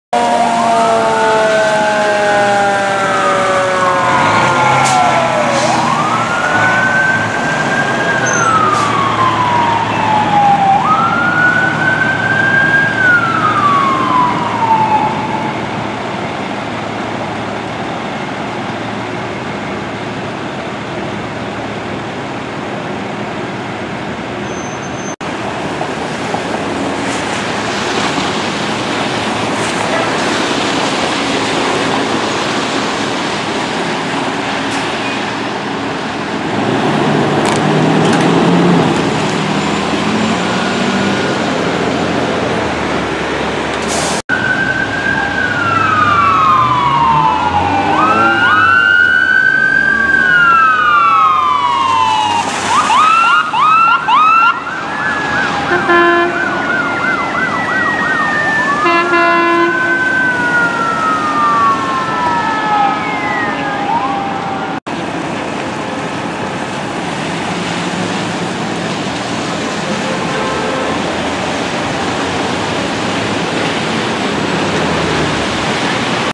LACoFD Engine 7, LAFD Engine 61 & Rescue 61 (reserve), & McCormick Ambulance 402
Los Angeles County Fire Department Engine 7, Los Angeles Fire Department Engine 61 and Rescue 61, and McCormick Ambulance 402 are seen arriving on scene of a traffic accident at San Vicente Boulevard and Beverly Boulevard. This incident occurred at the border of West Hollywood and Los Angeles, which is why both LA County and LA City responded to this incident.